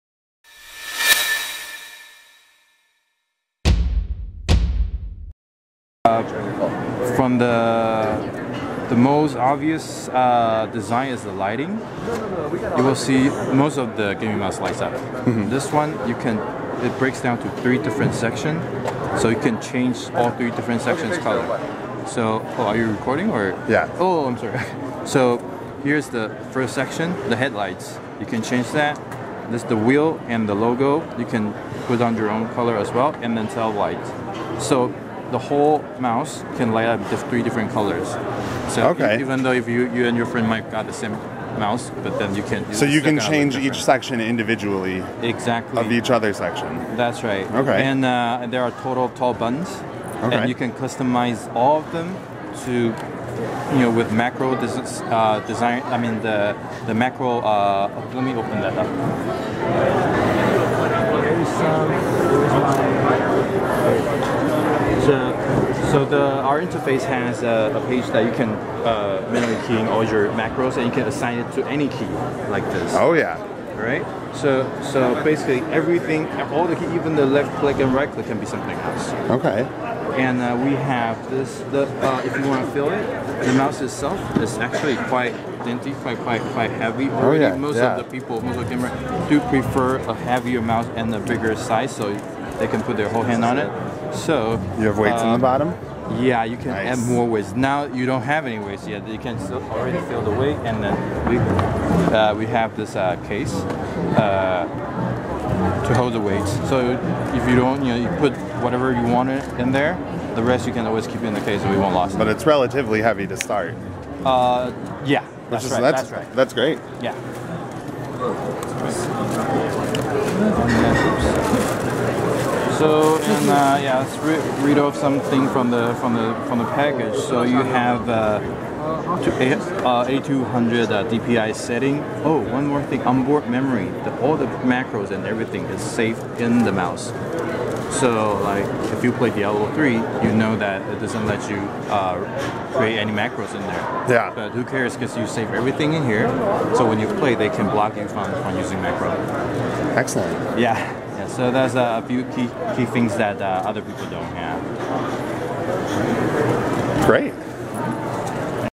Genius Gila Gaming Mouse Hands-On
We go hands-on with the Gila gaming mouse at CES 2013. Be sure to check out SlashGear.com for more coverage from the show!